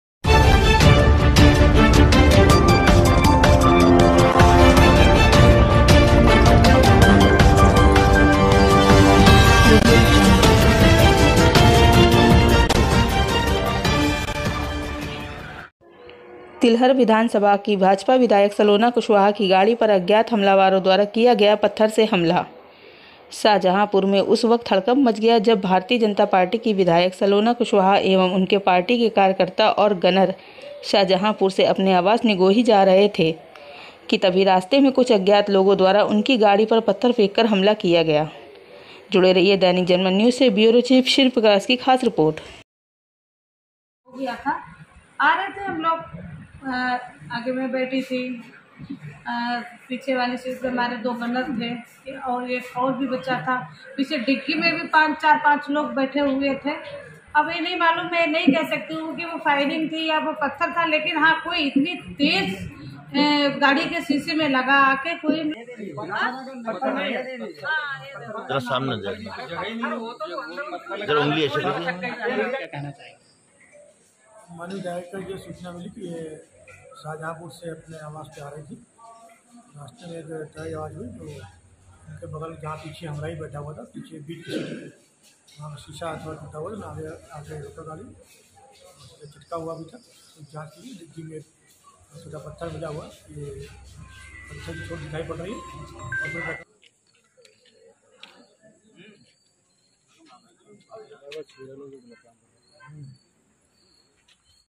तिलहर विधानसभा की भाजपा विधायक सलोना कुशवाहा की गाड़ी पर अज्ञात हमलावरों द्वारा पत्थर से किया गया?
तिलहर विधानसभा की भाजपा विधायक सलोना कुशवाहा की गाड़ी पर अज्ञात हमलावरों द्वारा पत्थर से किया गया हमला शाहजहांपुर में उस समय हड़कंप मच गया जब भारतीय जनता पार्टी की विधायक सलोना कुशवाहा एवं उनके पार्टी के कार्यकर्ता और गनर शाहजहांपुर से अपने आवास निगोही जा रहे थे कि तभी रास्ते में कुछ अज्ञात लोगों द्वारा उनकी गाड़ी पर पत्थर फेंक कर हमला किया जाना बताया जा रहा है, जिसकी सूचना पर पहुंची पुलिस ने छानबीन शुरु कर दी है, वही इस मामले में एसपी सिटी सुधीर जायसवाल ने बताया है, कि उक्त प्रकरण में जांच की जा रही है ।मामला भारतीय जनता पार्टी से तिलहर विधानसभा से विधायक सलोना कुशवाहा का है, जो आज रात्रि लगभग 7:00 बजे पर शाहजहांपुर से निगोही जा रही थी की तभी अचानक गिरिगिचा चीनी मिल के पास किसी पत्थर के टकराने एवं फायर की आवाज़ सुनकर सभी दहशत में आ गए, जहां इस सूचना से क्षेत्रीय जनता एवं विधायक समर्थकों में सनसनी फैल गई जिसके बाद चारों तरफ विधायकों के समर्थकों द्वारा विधायक पर हमला होने एवं फायर करने की चर्चाएं गर्म हो गई#Dainikjanmatnews
News 24